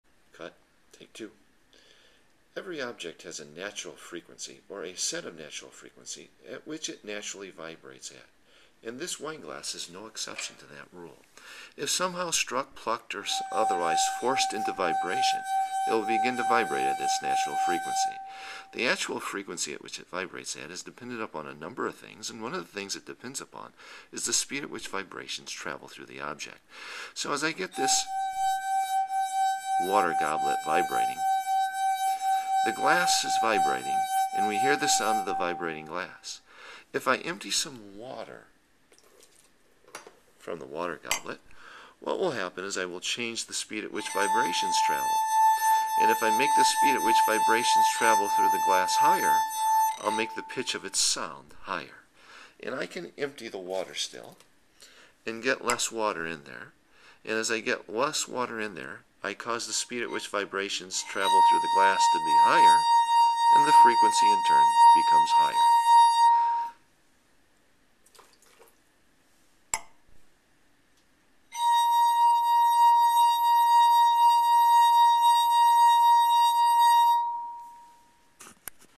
Water Goblet Resonance
A water goblet is forced to vibrate by running a finger over its rim. The stick-slip friction between the finger and the glass causes the glass to resonate in sync with the slipping and sticking of the finger. This forced vibration produces a distinct pitch. The pitch is then altered by removing water from the goblet, thus altering the speed of vibrations within the glass, and ultimately the frequency of its vibrations.